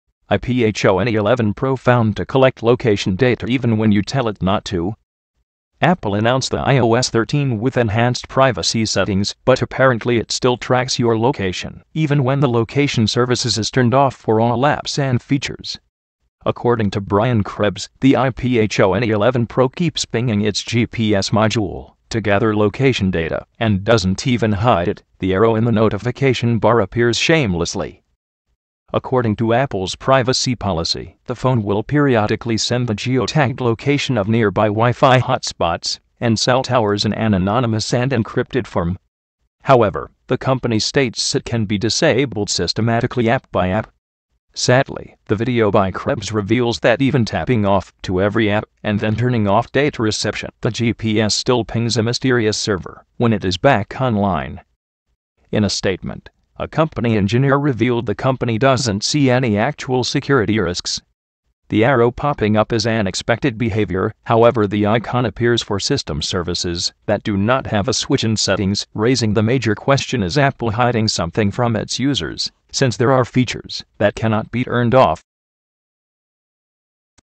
iPhone 11 Pro found to collect location data even when you tell it not to
Apple announced the iOS 13 with enhanced privacy settings, but apparently it still tracks your location, even when the Location Services is turned off for all apps and features. According to Brian Krebs, the iPhone 11 Pro keeps pinging its GPS module to gather location data and doesn’t even hide it - the arrow in the notification bar appears shamelessly

According to Apple’s Privacy policy, the phone will periodically send the geo-tagged location of nearby Wi-Fi hotspots and cell towers in an anonymous and encrypted form. However, the company states it can be disabled systematically app by app.

Sadly, the video by Krebs reveals that even tapping Off to every app, and then turning off data reception, the GPS still pings a mysterious server when it is back online

In a statement, a company engineer revealed the company doesn’t see any actual security risks. The arrow popping up is an “expected behavior”, however “the icon appears for system services that do not have a switch in Settings”, raising the major question is Apple hiding something from its users since there are features that cannot be turned off